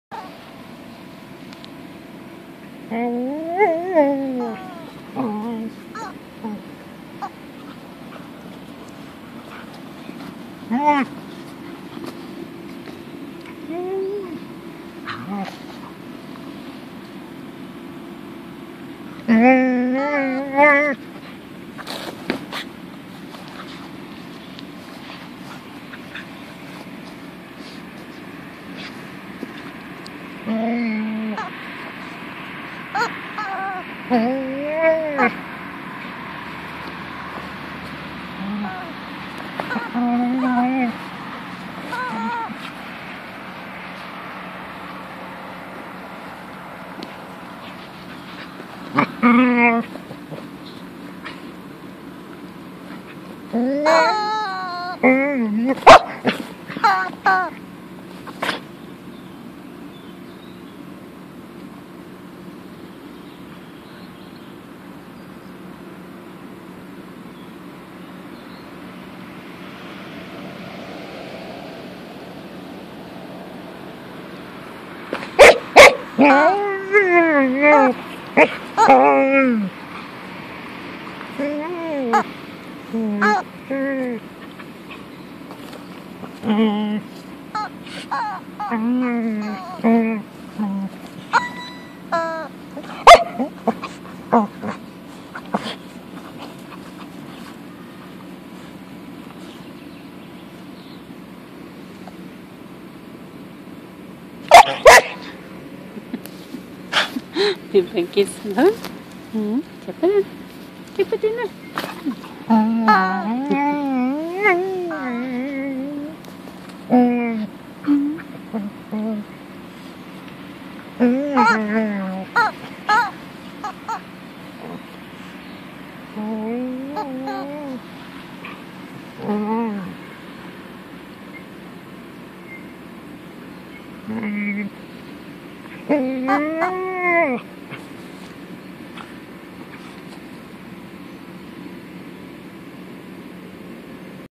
Funny dog talking to her chicken....
My funny dog Punky looooves her toychicken. Sometimes she`s just acting very wierd, but we love her too bits. She is my little angel girl.